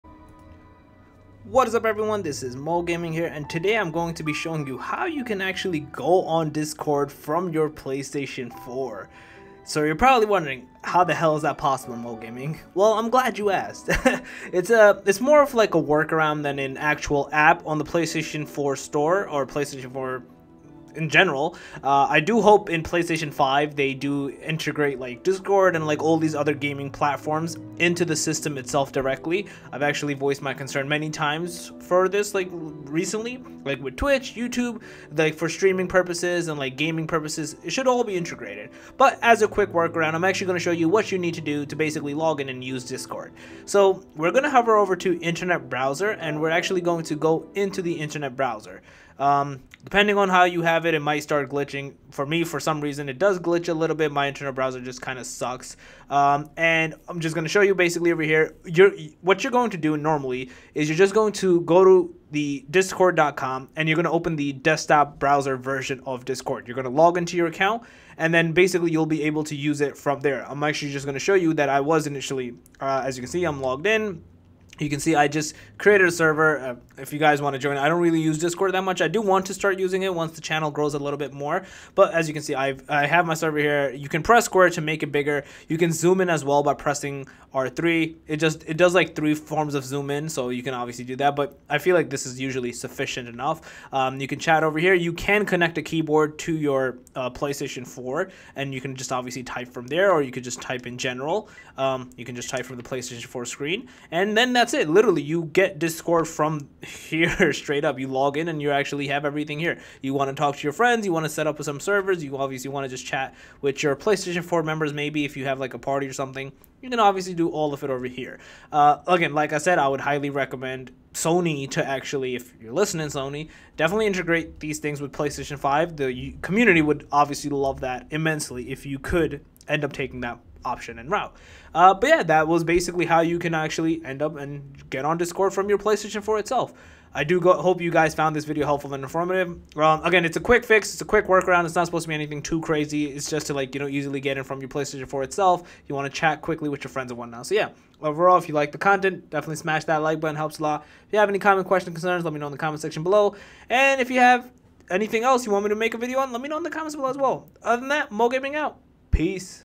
How To Use Discord App on Your PS4 2020 (100% Working!)
In this video i talk about How To Use Discord App on Your PS4 2020